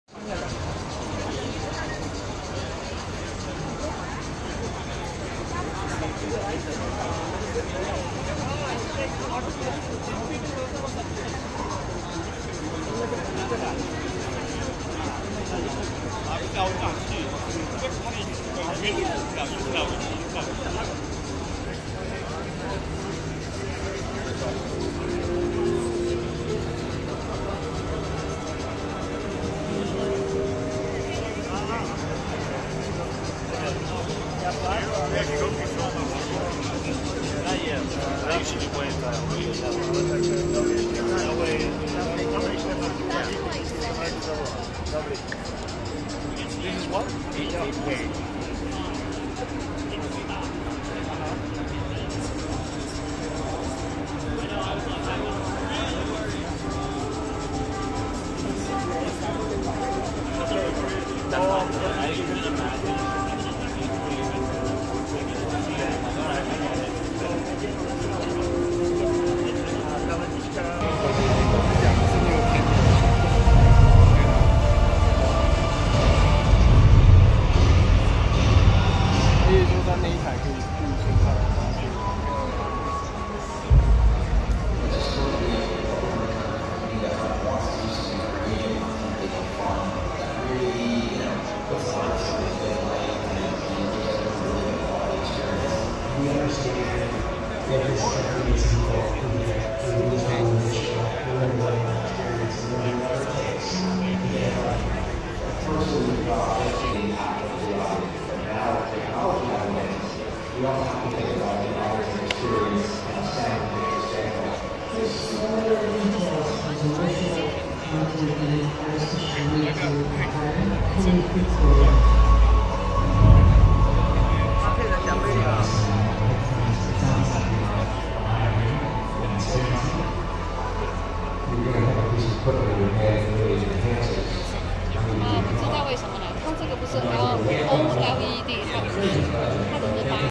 Sony Master Series Z9G 8K HDR 98" LED Android TV, CES 2019 [4K Video]
Sony - Z9G 8K TV - 98 inch
Master Series 8K HDR X1 Ultimate Processor
Android TV

Video taken at CES 2019 from Sony booth.

All content in this video is owned by SONY CORPORATION OF AMERICA.